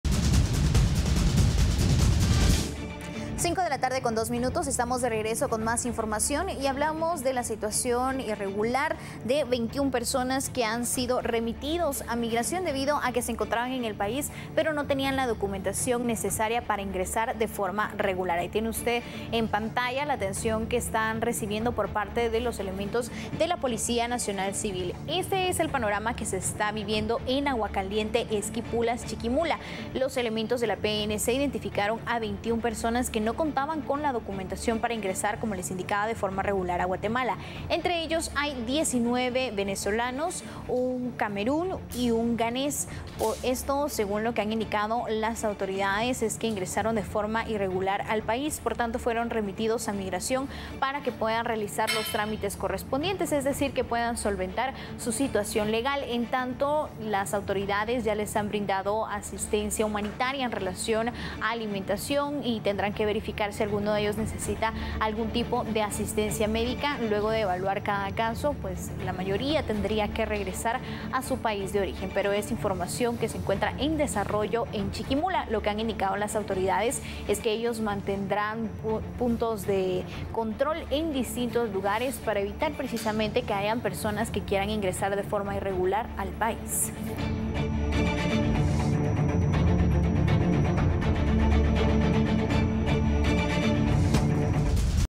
21 personas remitidas de Esquipulas a migración
21 personas remitidas de Esquipulas a migración.